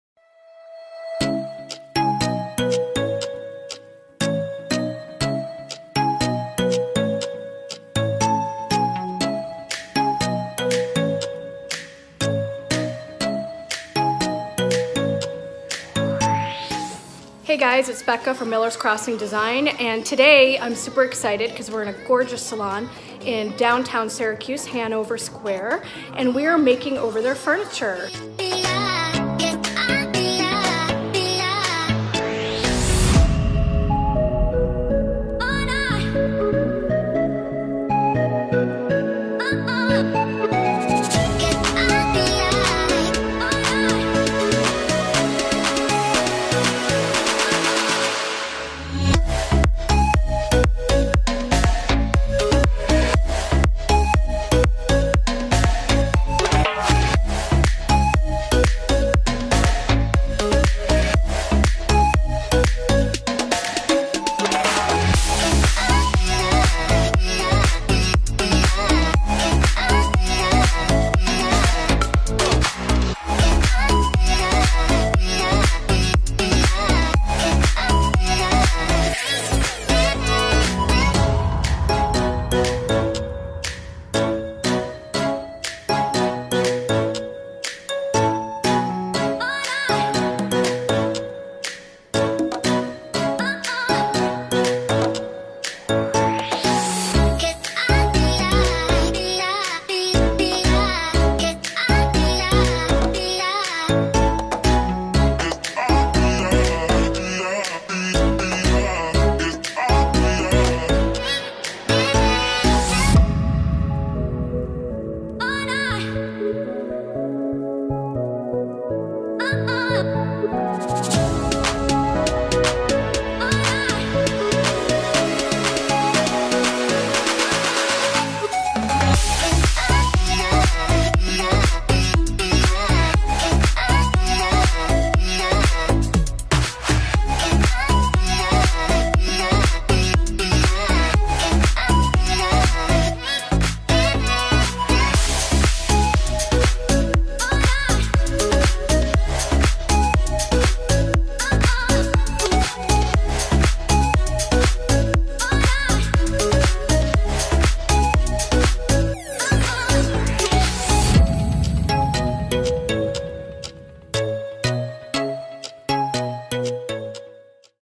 Salon Station Makeover Vlog
Miller's Crossing Design was asked by Artistry in Hanover Salon to makeover their salon stations! This video is a sweet, short, and fun little vlog of the transformation on these pieces. Enjoy!